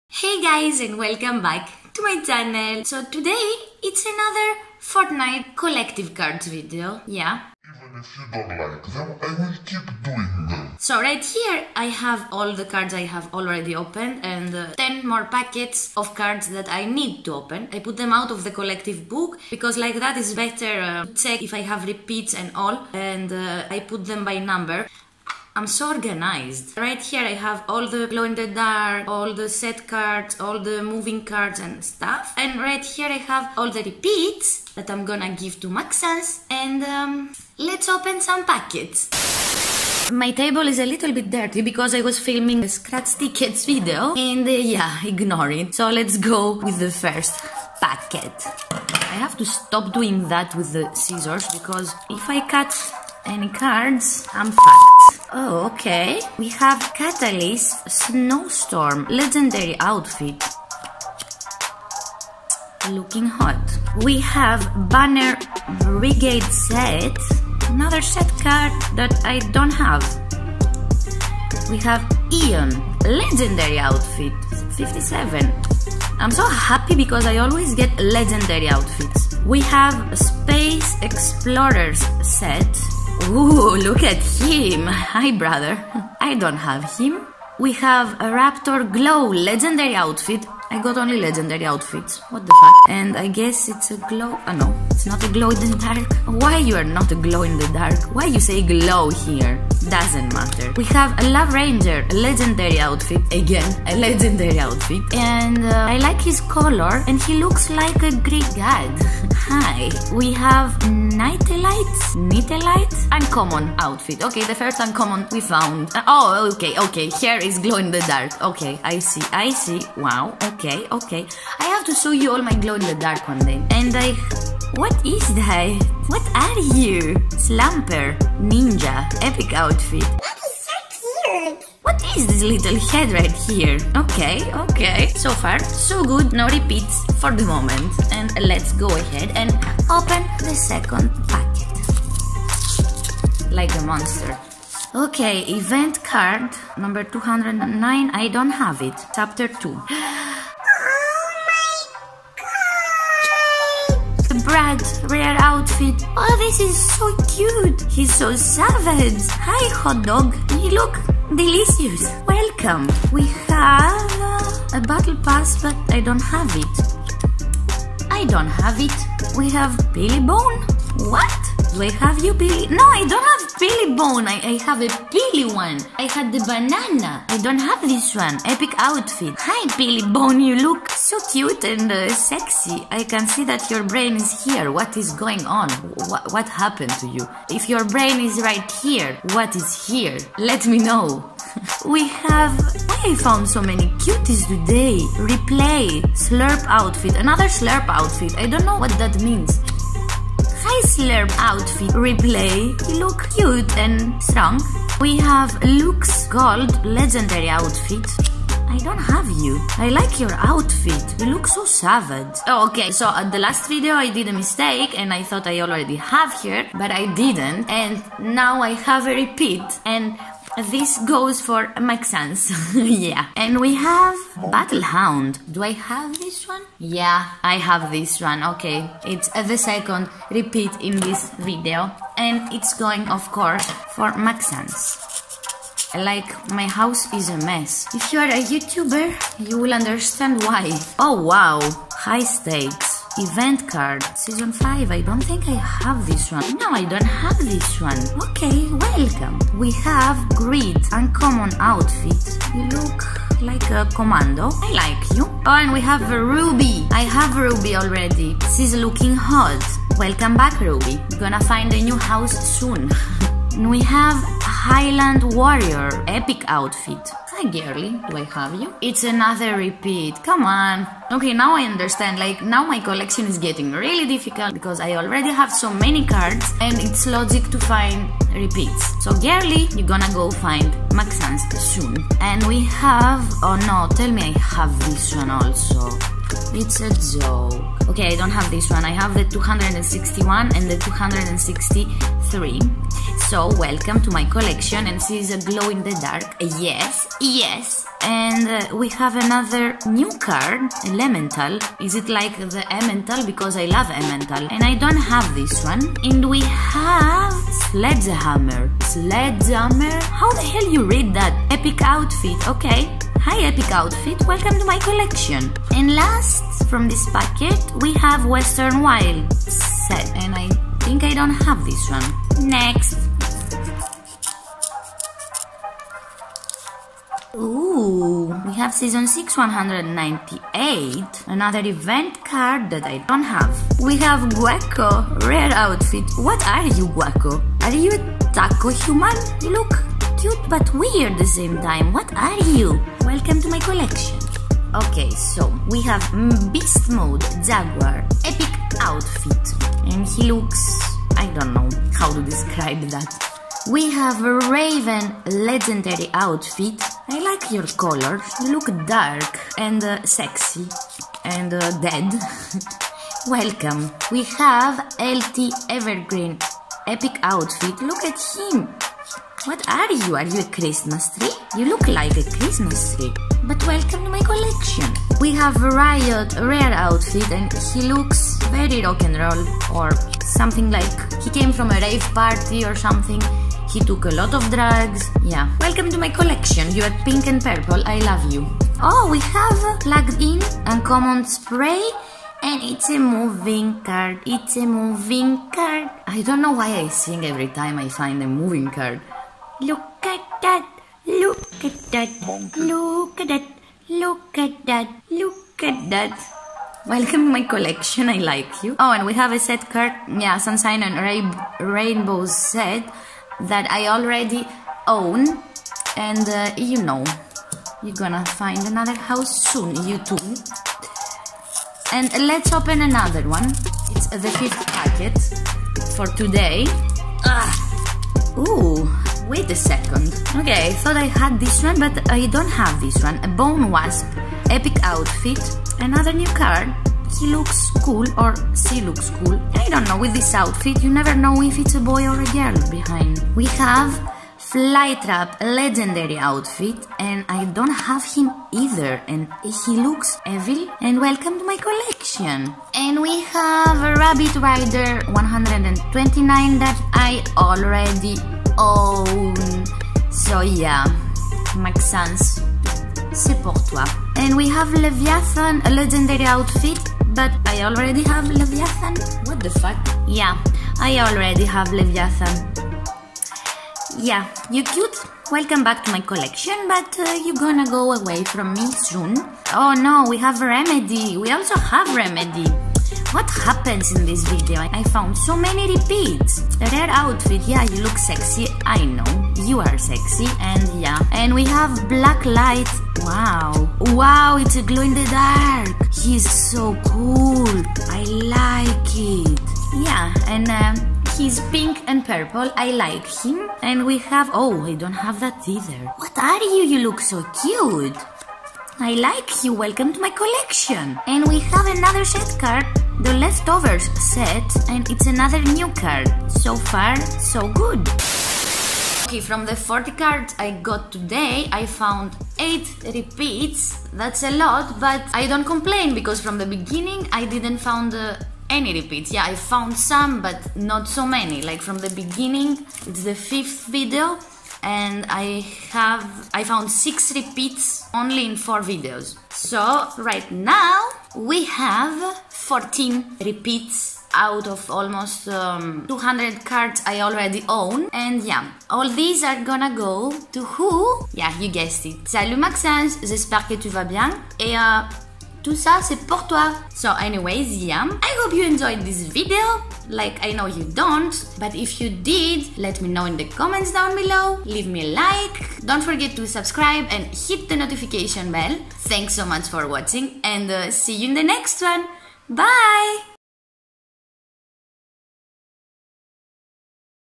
Panini FORTNITE Reloaded (2020) COLLECTIVE CARDS (PART 4)
Hey fam!
My collection is getting bigger and bigger!
I had a problem with my editing program and my video is not so good!